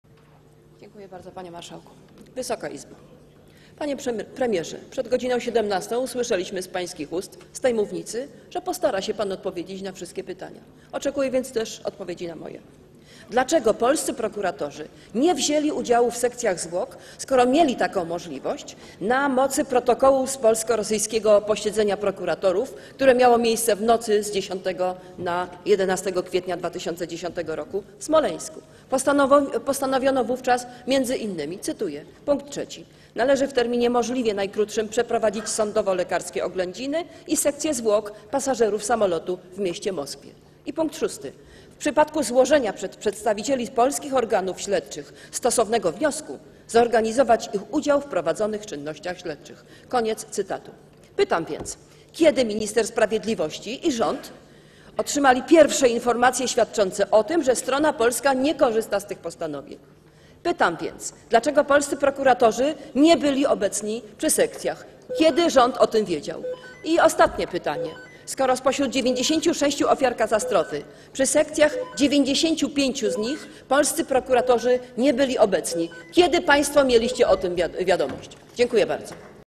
[374/454] Anna Paluch: Panie Premierze! Przed godz. 17 usłyszeliśmy z pańskich ust z tej mówn...
Posiedzenie Sejmu nr 22
27 września 2012 r. 19:32:08
Poseł Anna Paluch:

Dziękuję bardzo.  Panie Marszałku!  Wysoka Izbo!  Panie Premierze!  Przed godz.

17 usłyszeliśmy z pańskich ust z tej mównicy, że postara się pan odpowiedzieć na wszystkie pytania.  Oczekuję więc też odpowiedzi na moje.  Dlaczego polscy prokuratorzy nie wzięli udziału w sekcjach zwłok, skoro mieli taką możliwość na mocy protokołu z polsko-rosyjskiego posiedzenia prokuratorów, które miało miejsce w nocy z 10 na 11 kwietnia 2010 r.  w Smoleńsku?  Postanowiono wówczas między innymi: pkt 3: „należy w terminie możliwie najkrótszym przeprowadzić sądowo-lekarskie oględziny i sekcję zwłok pasażerów samolotu w mieście Moskwie", pkt 6: „w przypadku złożenia przez przedstawicieli polskich organów śledczych stosownego wniosku zorganizować ich udział w prowadzonych czynnościach śledczych".

Pytam więc: Kiedy minister sprawiedliwości i rząd otrzymali pierwsze informacje świadczące o tym, że strona polska nie korzysta z tych postanowień?  Pytam więc: Dlaczego polscy prokuratorzy nie byli obecni przy sekcjach?

(Dzwonek)

Kiedy rząd o tym wiedział?  I ostatnie pytanie: Skoro na 96 oﬁar katastrofy przy sekcjach 95 z nich polscy prokuratorzy nie byli obecni, kiedy państwo mieliście o tym wiadomość?  Dziękuję bardzo.

(Oklaski)

(Przewodnictwo w obradach obejmuje marszałek Sejmu Ewa Kopacz)